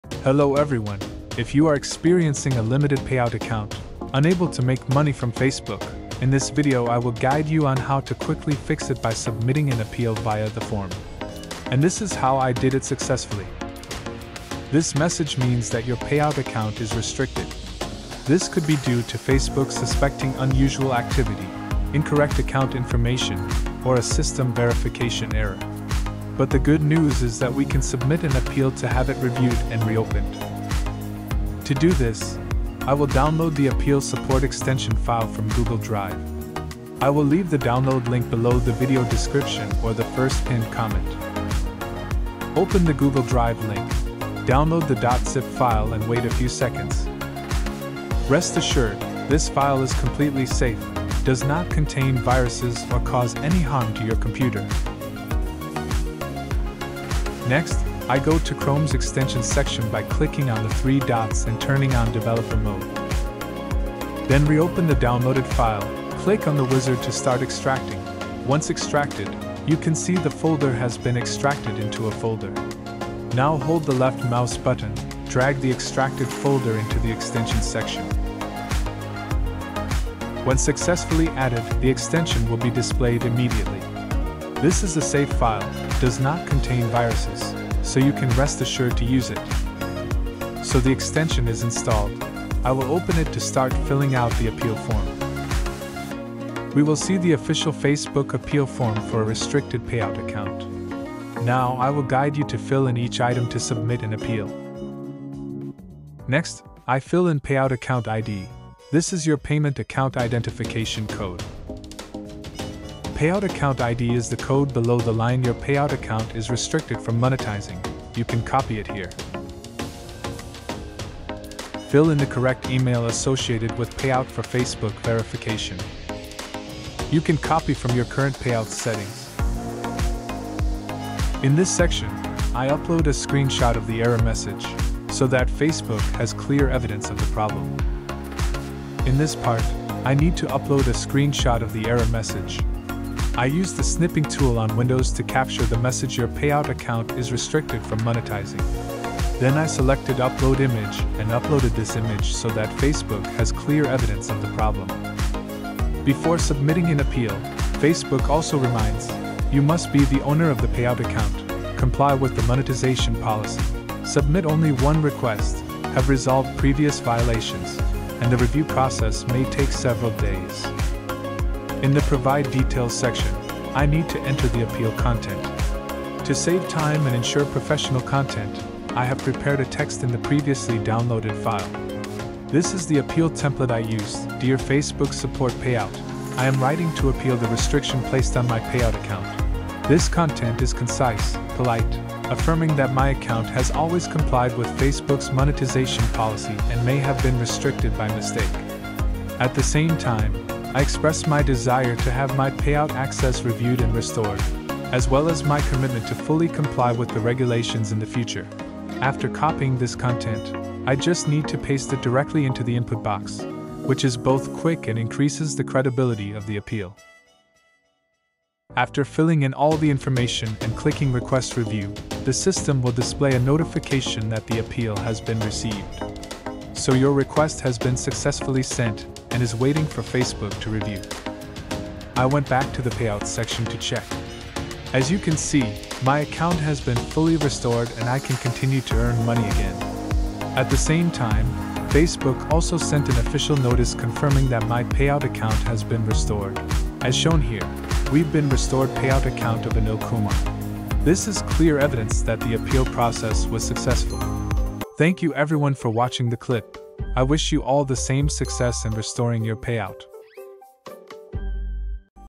How to Fix "Your Payout Account is Restricted from Monetising" on Facebook (2026 Update)
In this video, I’ll walk you step by step through the exact process to fix and recover a restricted Facebook payout account.

✨ This is the latest working solution for 2026, personally tested and successfully used to restore my own payout account and resume monetization.

🔹 How to fix Facebook payout account restrictions step by step
🔹 How to submit your appeal correctly to Facebook
🔹 How to restore your payout account and start monetizing again
🔹 The correct appeal submission process (no mistakes)
🔹 How to confirm when Facebook has fully restored your payout account

🚀 Who This Tutorial Is For

✔️ Creators with monetization disabled
✔️ Business pages with payout restrictions
✔️ Anyone restricted from monetizing on Facebook
✔️ Users who want a safe and official recovery method

✨ Important
If your payout account is restricted, follow this tutorial carefully and apply the same steps to fix the issue quickly and safely without using risky tools or paid services.